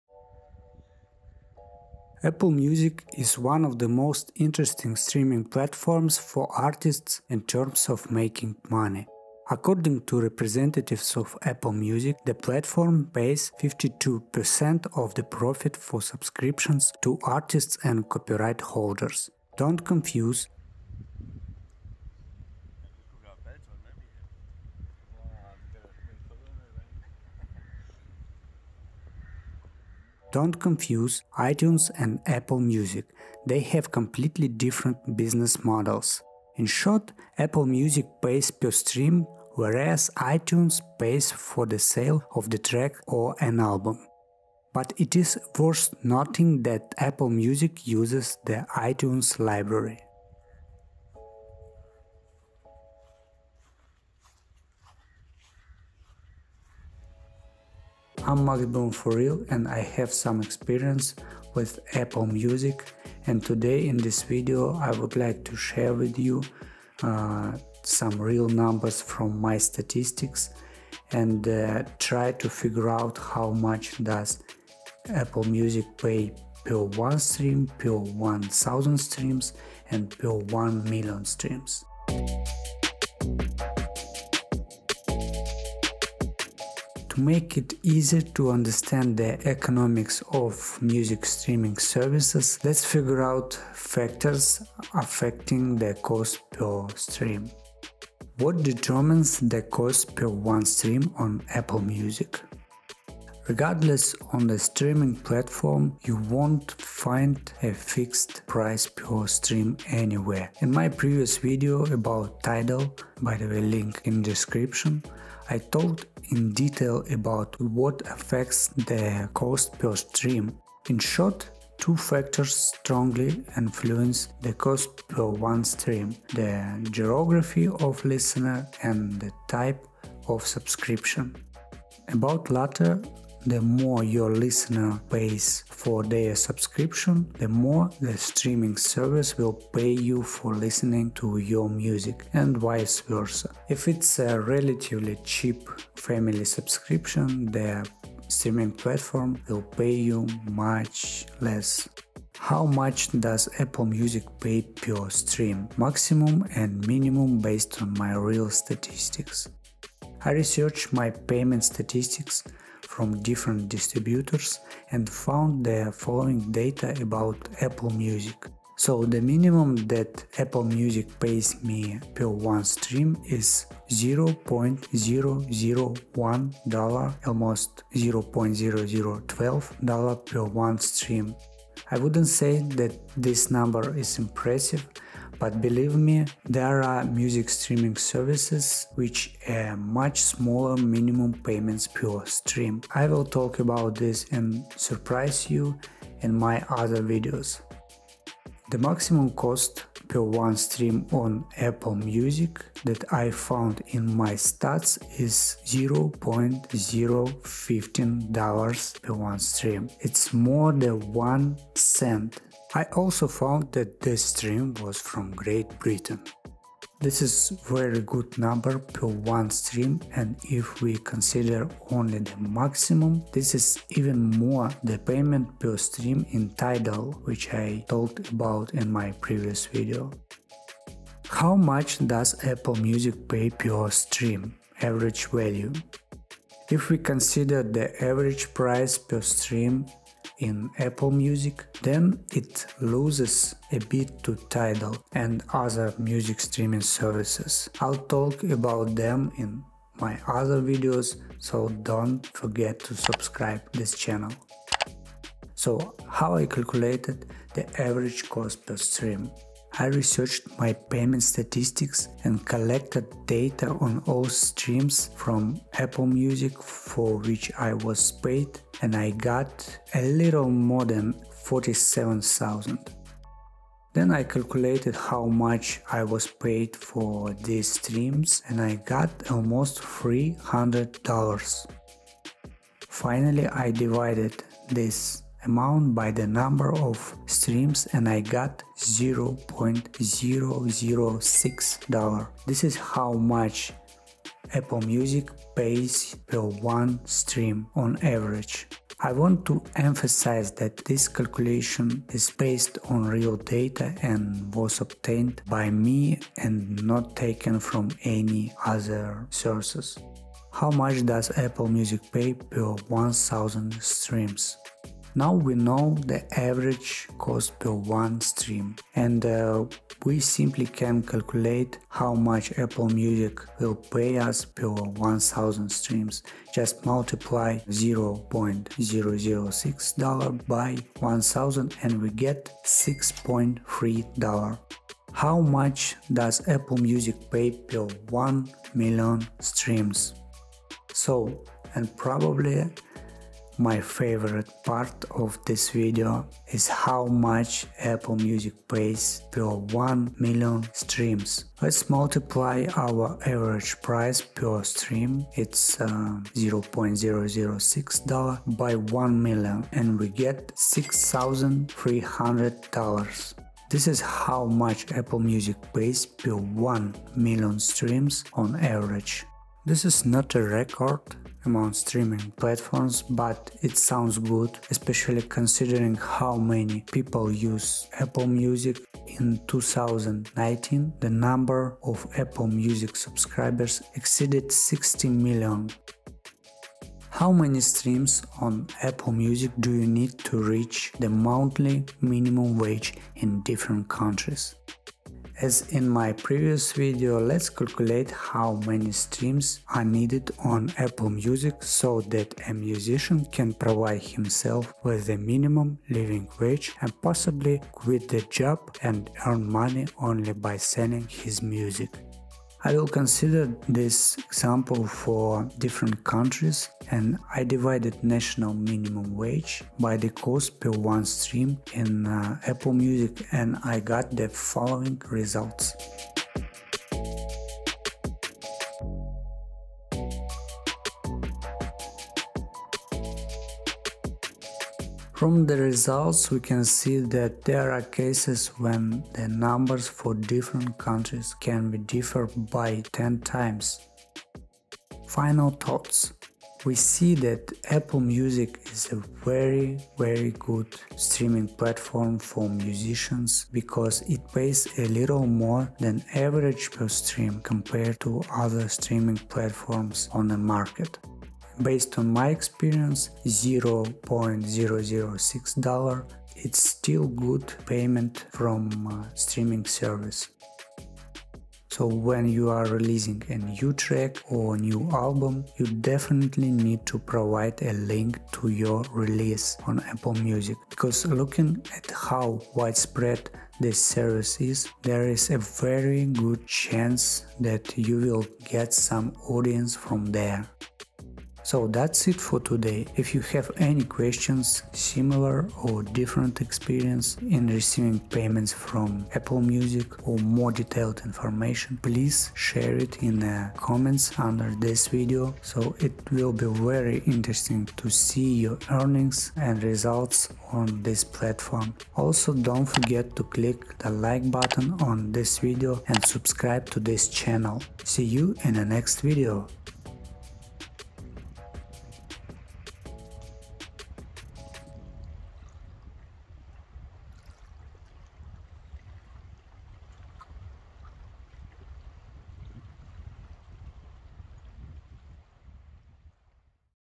How Much Does Apple Music Pay Per Stream, Per 1000 & for 1 Million Streams 💰
💲 In this video, I share with you how much Apple Music pays per stream based on my real payment statistics from Apple Music for streaming (listening/plays) my music on the platform. In the video, I show you how much money Apple Music pays per 1 stream, per 1000 streams, and calculate the revenue you can earn from Apple Music for 1 million streams based on the average stream cost.

💰 Apple Music is a good service for you as a music artist to make money from your music (especially considering how widespread it is), but it's payments per stream are not the highest among all music streaming platforms.

🕒 Chapters:
[0:00] Intro
[1:37] What determines the cost per 1 stream on Apple Music?
[2:33] How much does Apple Music pay per stream? (maximum and minimum)?
[3:50] How much does Apple Music pay per stream? (average)?
[5:10] How much does Apple Music pay per 1000 streams?
[5:35] How much does Apple Music pay for 1 million streams?
[6:26] How many streams on Apple Music do you need to reach the monthly minimum wage (NMW) in different countries
[7:25] Final Thoughts
[8:10] Outro